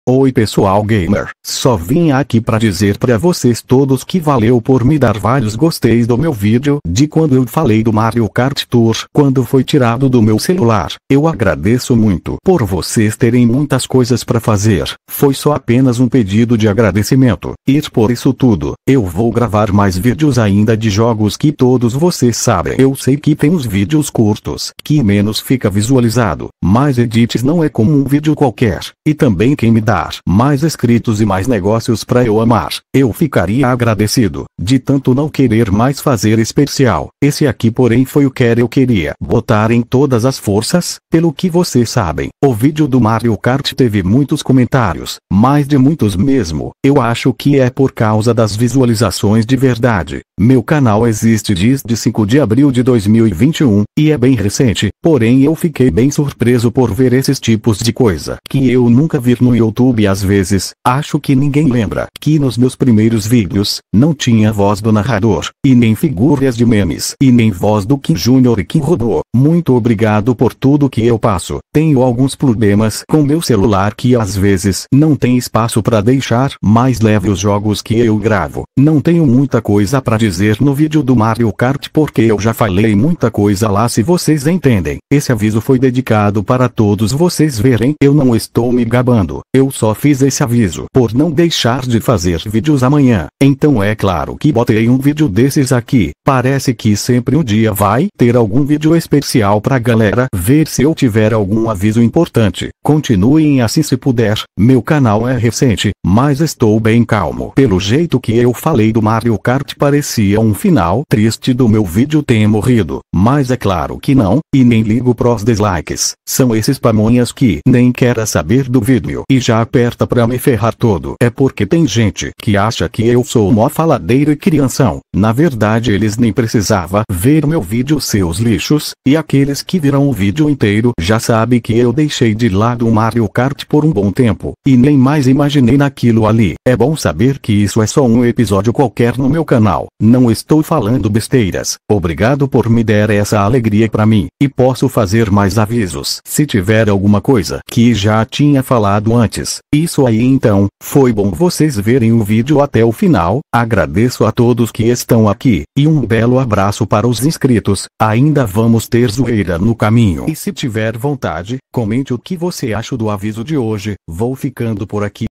Aviso de Agradecimento Do vídeo do Mario Kart: (curtidas, e Falando sobre o Resto)
obrigado por me dar valhas opções, Eu agradeço muito por me der várias curtidas nesse vídeo qui faz semanas, tchau a todos e tenham um bom descanso.